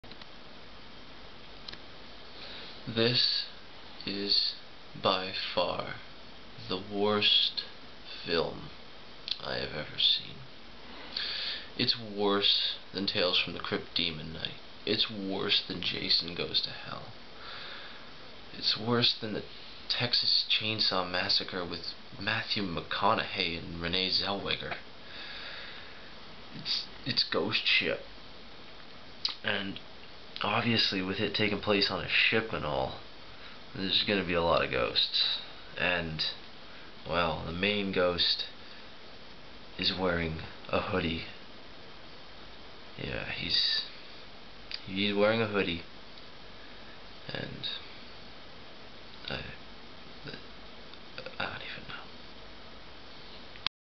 Video Vault #2: Ghost Ship (2002)
Video Vault makes its way out to SEA this terrible movie.